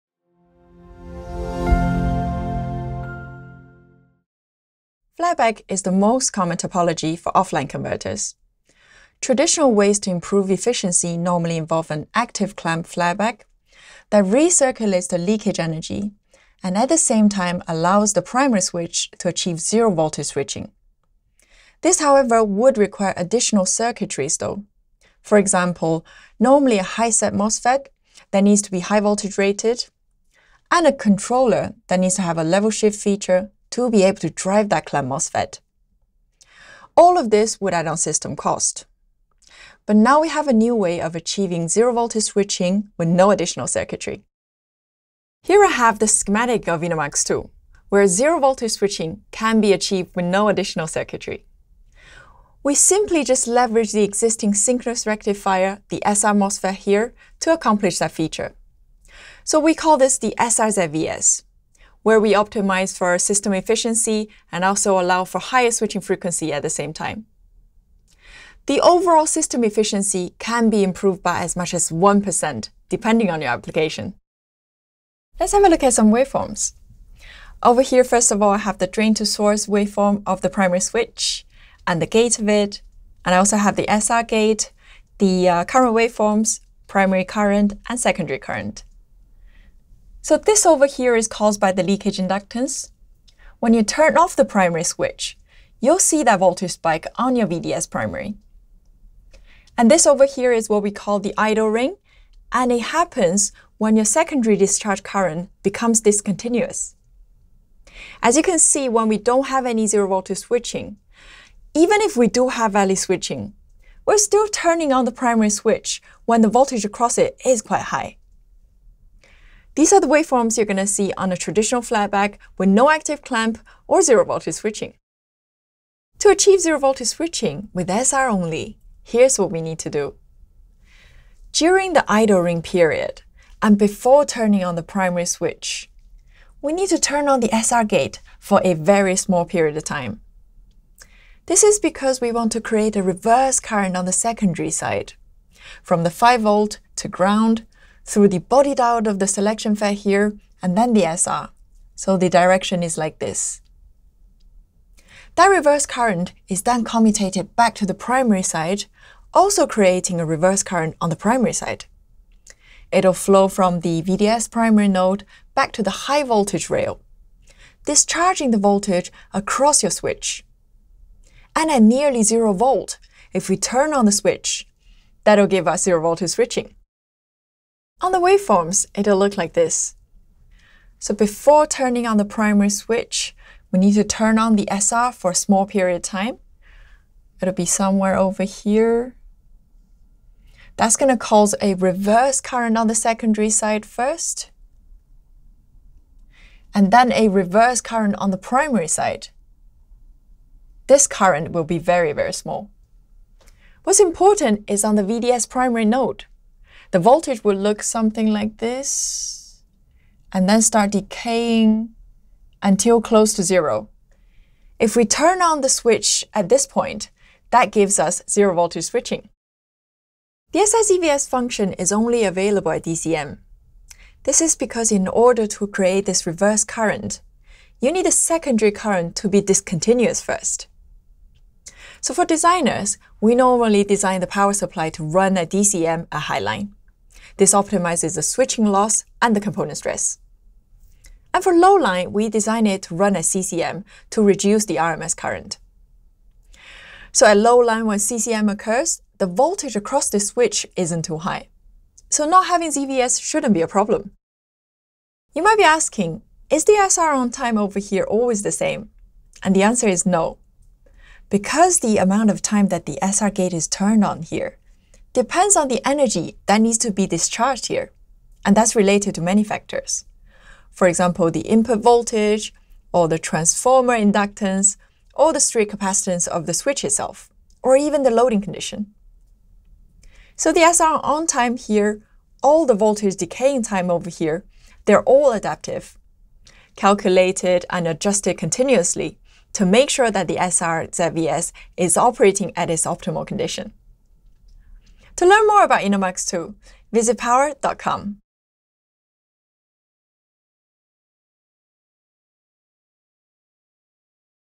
InnoMux-2 - Zero Voltage Switching Without Active Clamp
Highly integrated, highly efficient InnoMux-2 switcher ICs for multi-output flyback power supplies achieve zero voltage switching (ZVS) without the need for active clamping. Senior Applications Engineer Han Cui explains how it is done.